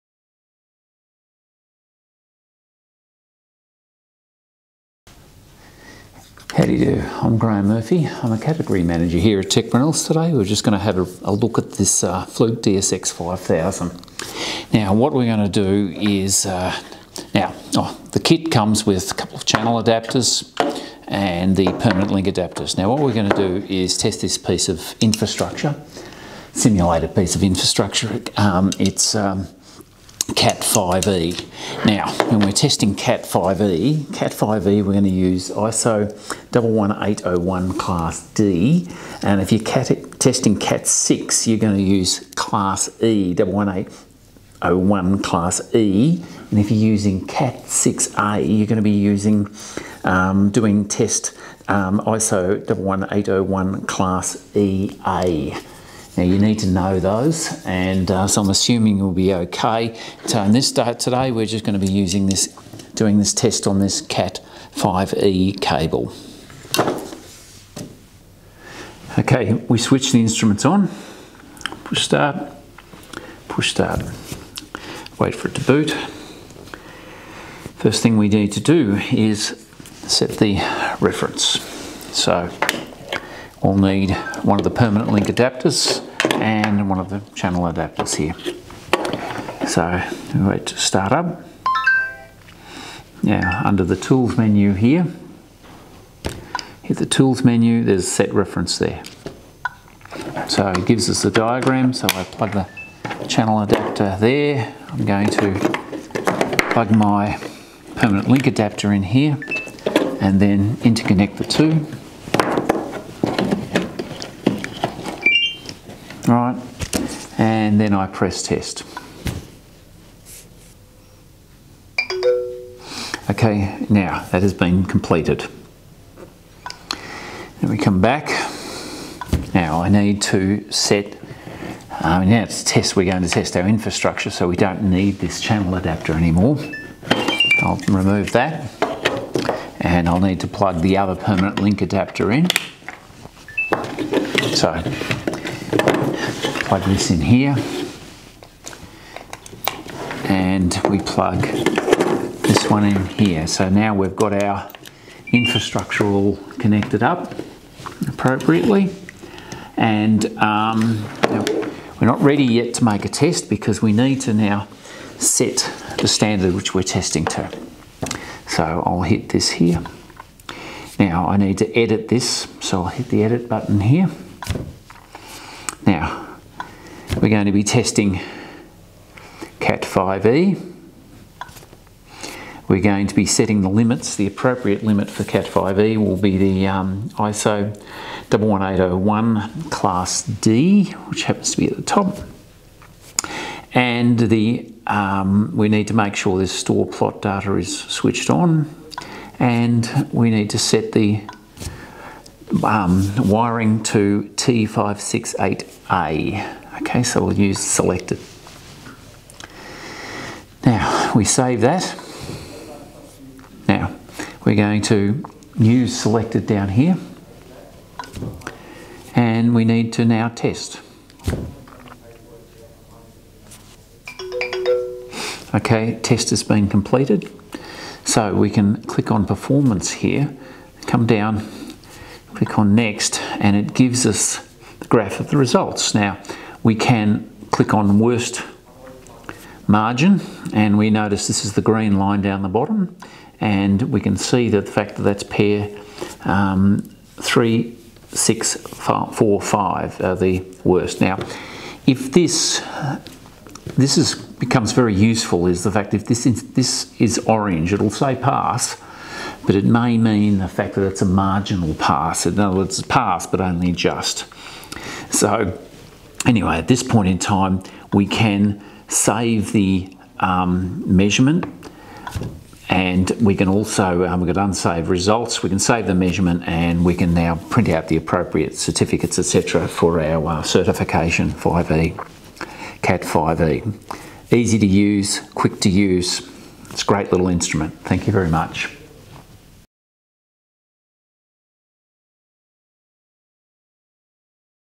Cable Testing with the Fluke DSX-5000
This video demonstrates how to use the Fluke DSX-5000 to test the integrity of a line of cable.